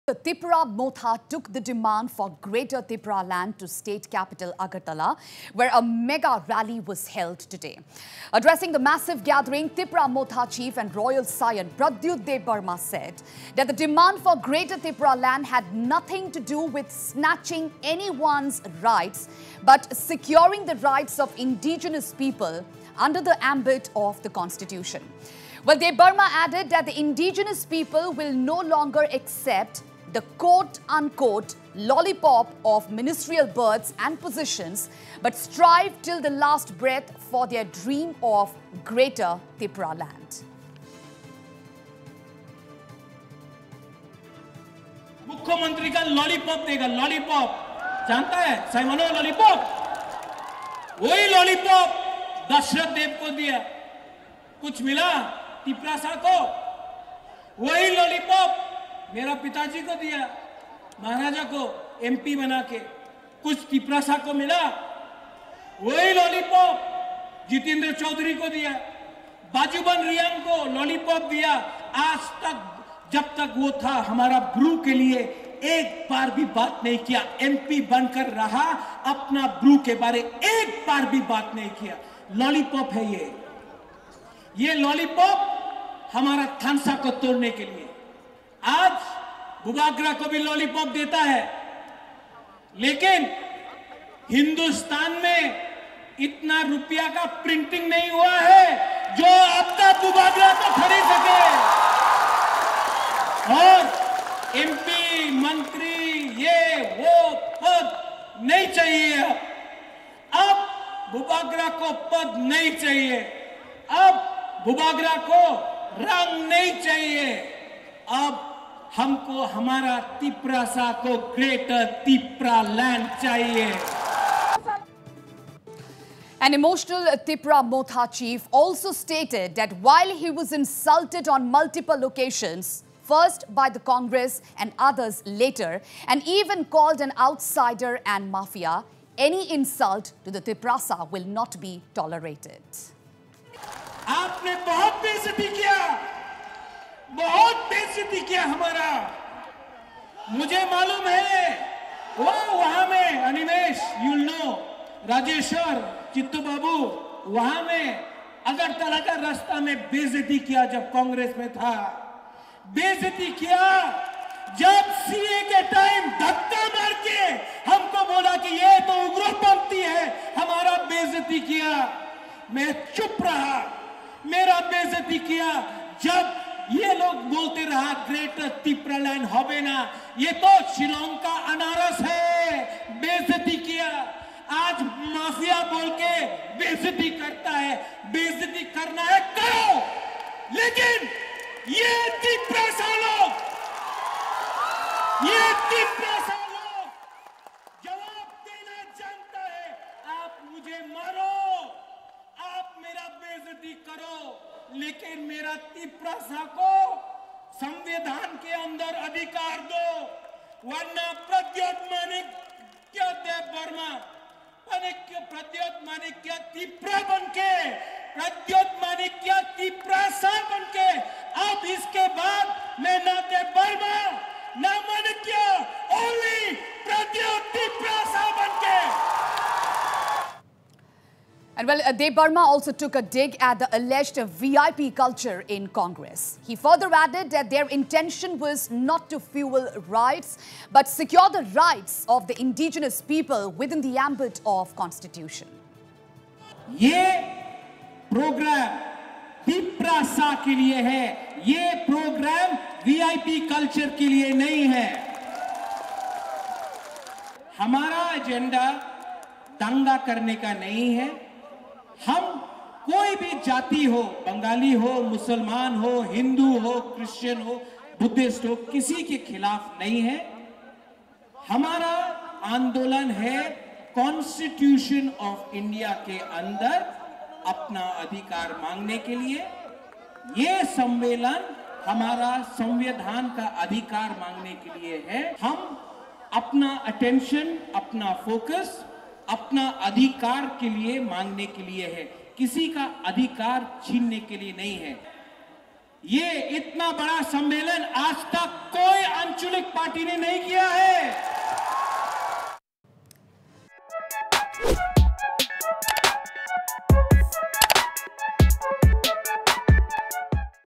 Tripura | Mega Tipraland Rally rocks Agartala
Tripura |The Tipra Motha took the demand for Greater Tipraland to state capital Agartala where a mega rally was held today.

A fierce Pradyot Bikram Manikya DebBarma said that the indigenous people will no longer accept the quote unquote lollipop of ministerial berths and positions but strive till the last breath for their dream of Greater Tipraland.